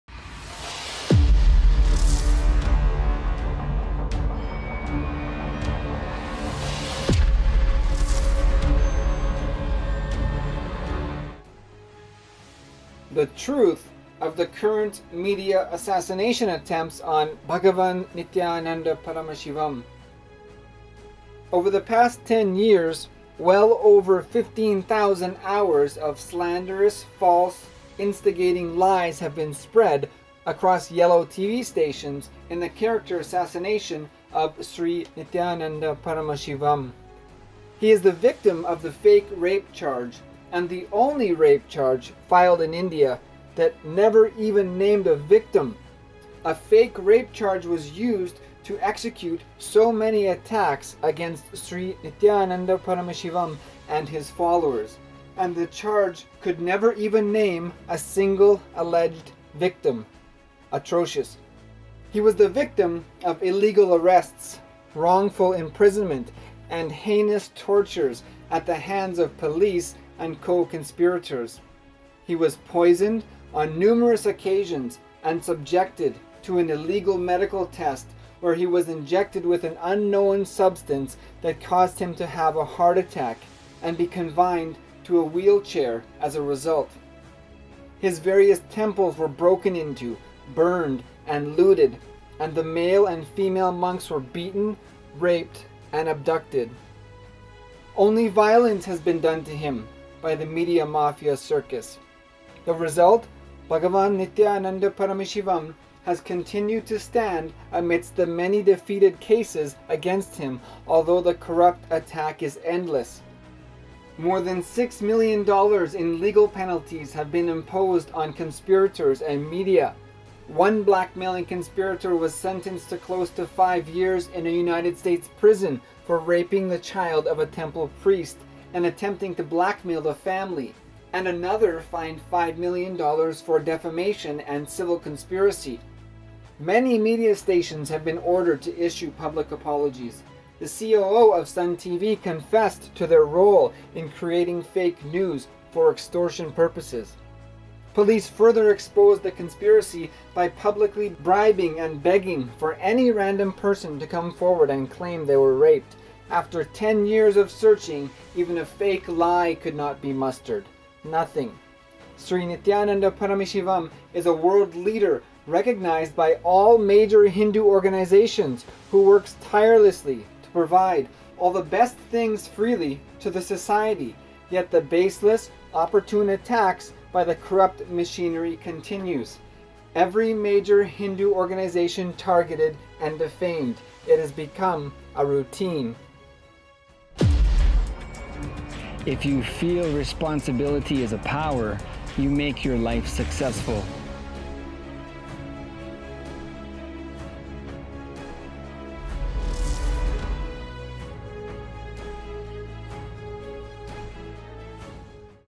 Leaders Targeted - Hinduism Now, 24 November 2019
The truth of the current media assassination attempts on Bhagavan Nithyananda Paramashivam:
Over the past 10 years, well over 15,000 hours of slanderous, false, instigating lies have been spread across yellow tv stations in the character assassination of Sri Nithyananda Paramashivam.
He is the victim of the fake rape charge, and the only rape charge filed in India that never even named a victim.  A fake rape charge was used to execute so many attacks against Sri Nithyananda Paramashivam and his followers, and the charge could never even name a single alleged victim - atrocious.
He was the victim of illegal arrests, wrongful imprisonment and heinous tortures at the hands of police and co-conspirators.  He was poisoned, on numerous occasions and subjected to an illegal medical test where he was injected with an unknown substance that caused him to have a heart attack and be confined to a wheel chair as a result.  His various temples were broken into, burned, and looted and the male and female monks were beaten, raped, and abducted.
Only violence has been done to him by the Mediamafia Circus.
The Result:
Bhagavan Nithyananda Paramashivam has continued to stand amidst the many defeated cases against him, although the corrupt attack is endless.
More than USD$6 Million in legal penalties have been imposed on conspirators and media.
One blackmailing conspirator was sentenced close to 5 years in a United States prison for raping the child of a Temple priest and attempting to blackmail the family and another fined $5 million for defamation and civil conspiracy.  Many media stations have been ordered to issue public apologies.   The COO of Sun TV confessed to their role in creating fake news for extortion purposes.
Police further exposed the conspiracy by publicly bribing and begging for any random person to come forward and claim they were raped.  After 10 years of searching even a fake lie could not be mustered, nothing.
Sri Nithyananda Paramashivam is a world leader recognized by all major Hindu organizations who works tirelessly to provide all the best things freely to the society yet the baseless, opportune attacks by the corrupt machinery continues.  Every major Hindu organization targeted and defamed, it has become a routine.